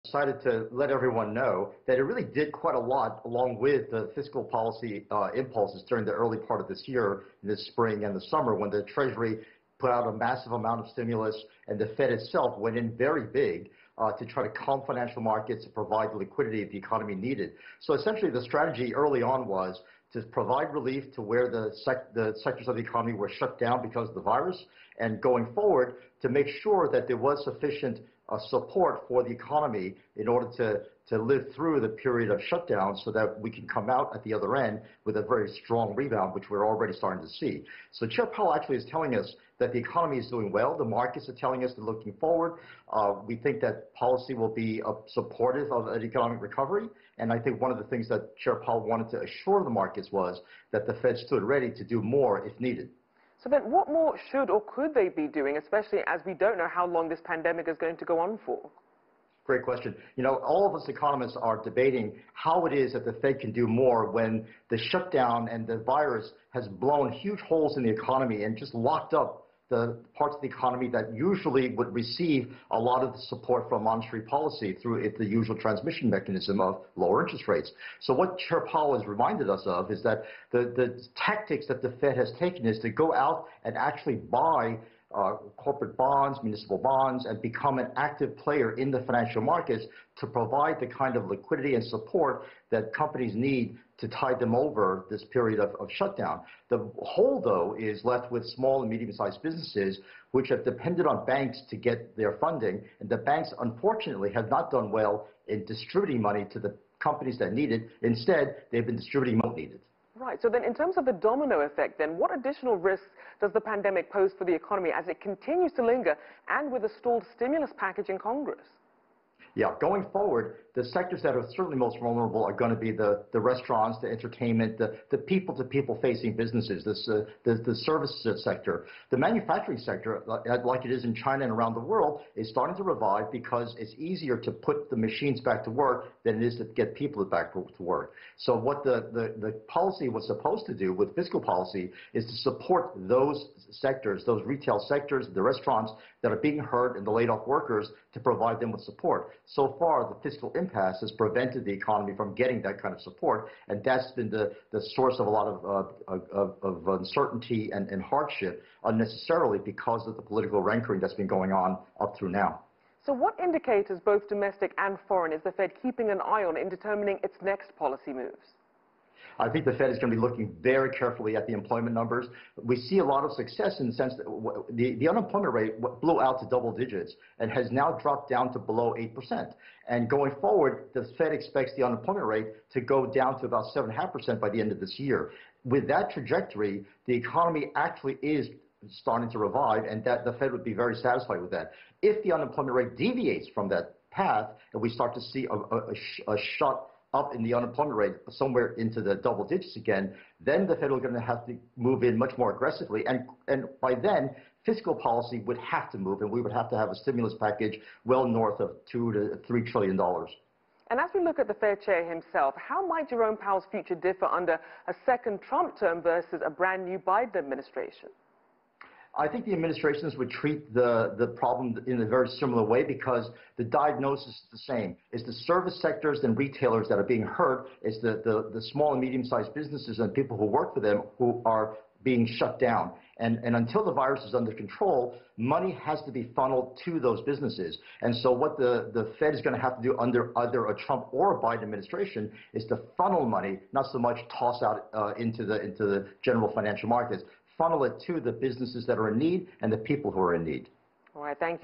William Lee on US decision to keep interest rates near zero
CGTN’s Rachelle Akuffo spoke to William Lee, chief economist at Milken Institute, about the US Federal Reserve’s economic plan amid pandemic and election.